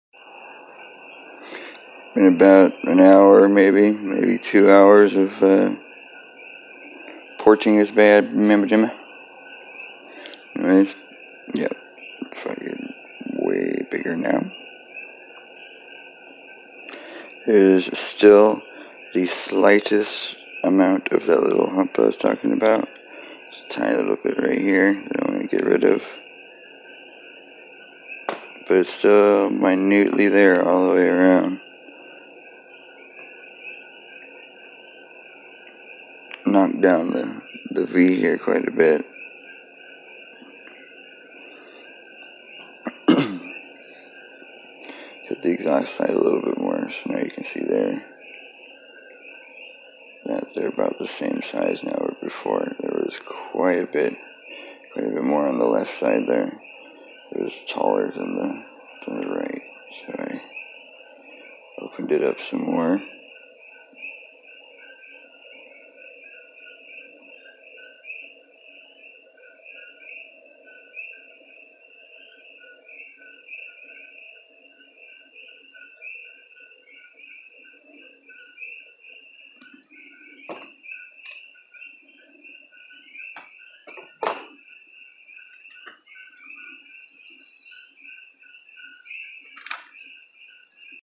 Porting my rmz 250 head up to 290 flow
I feel after watching videos of other porters I feel what ive done is going to complement the stage two cams and 290 bore kit. One guy said they all play there part . I have two more things I want to do to the head than all show a update .
Im going to shoot a video of braking everything down , than of my diy method of taking out my valve retaners without half the tool requires.. Starting now haha.